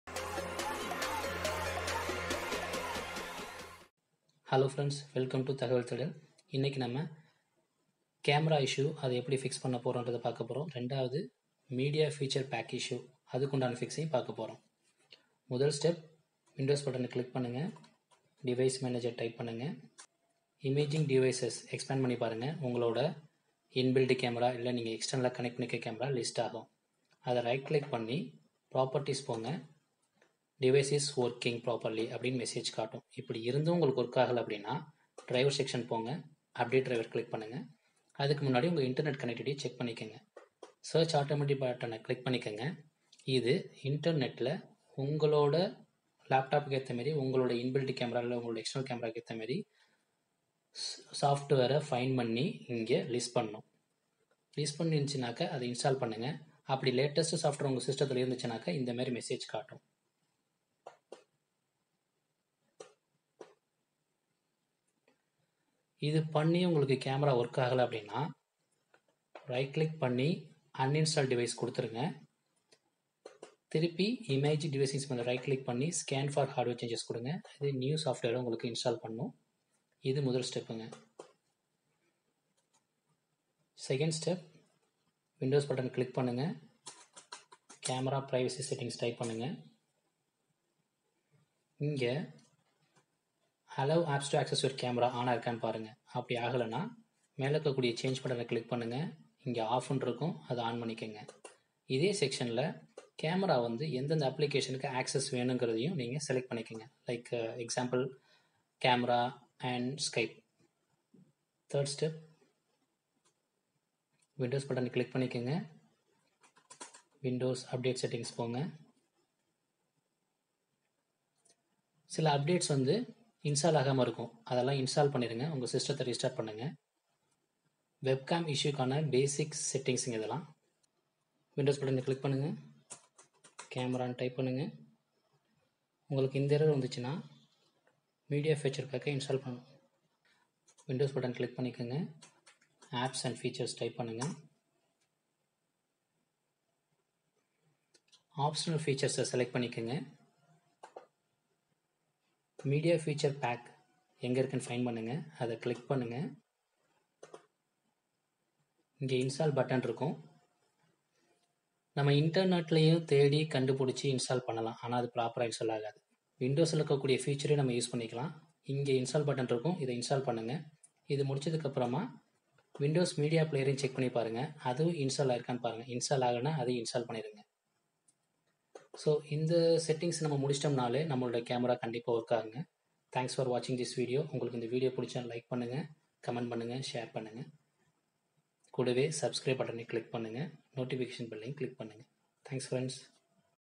How to fix camera issue | windows | media feature pack N and KN | tamil | Thagaval Thedal
Explained, how to fix webcam issues in Tamil, In this video, will show you how to test webcam to see if it is working. Make sure your applications have permission to use your camera on your computer. If your camera don't show up under device manager, scan for new hardware. If they still don't show up, there could be a connection issue with your computer and the device. Plug the device into a different USB port. | Thagaval Thedal

1. Device Manager - Imaging devices - Scan for hardware changes and do driver updates

2. Camera Privacy Settings- Make sure to "ON" Allow apps to access your Camera, provide permission to required application like Camera, Skype

3. Apps & Features - optional features - Install Media Feature Pack. if Windows Media Player not installed, Please that too.

4. Windows update - Make sure to update all pending list of software and do system restart.